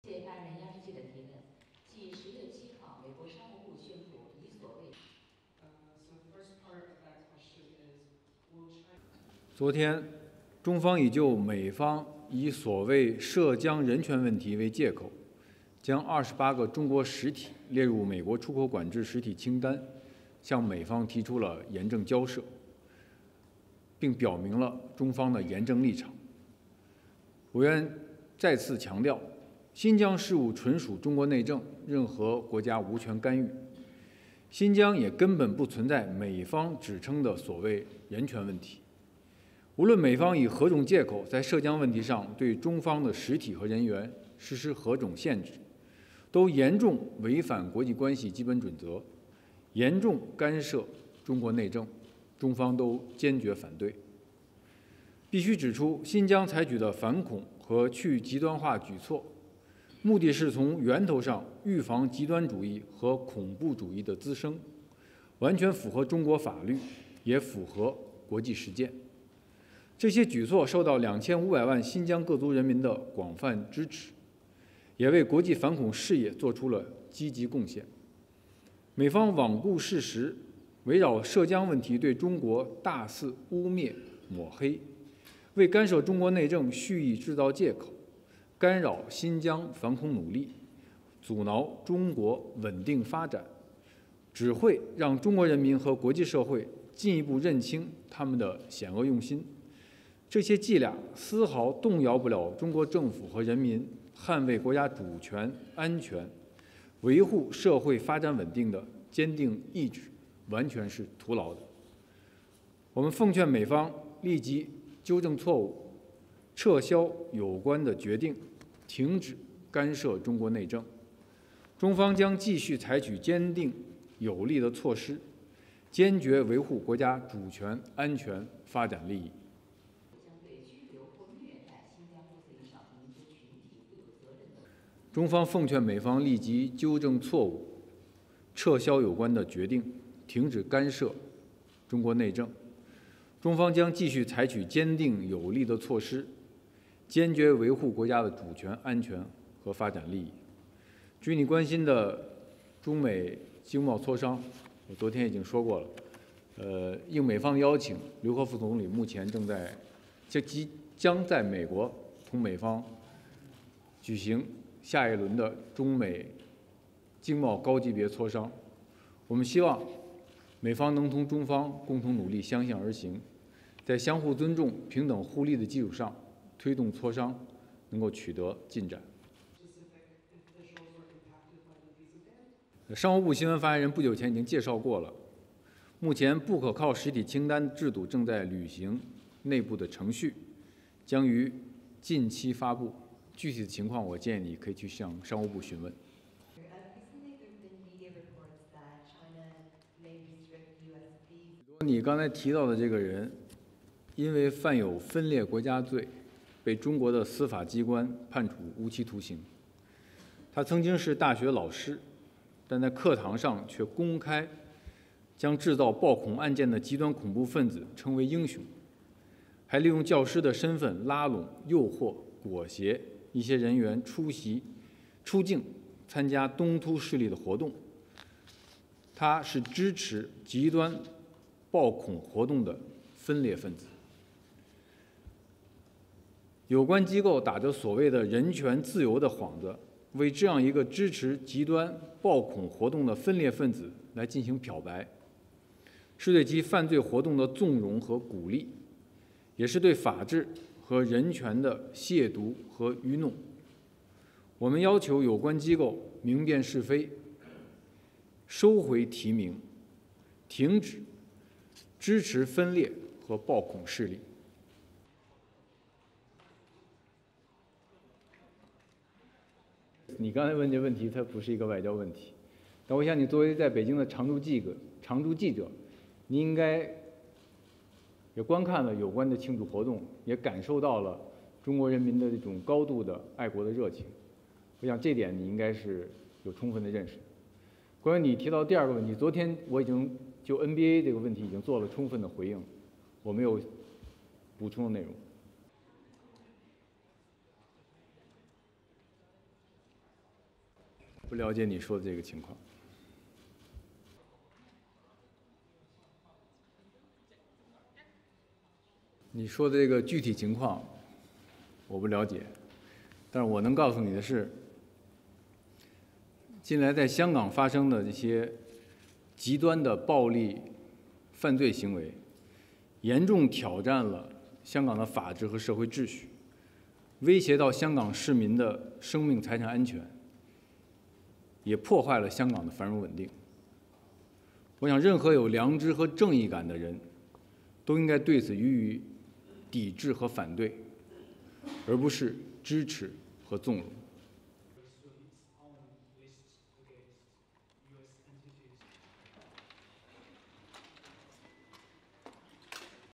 DAILY MOFA BRIEFING
(9 Oct 2019) MOFA Briefing
Spokesperson: Geng Shuang
Date: 9 October 2019
Duration: 05:54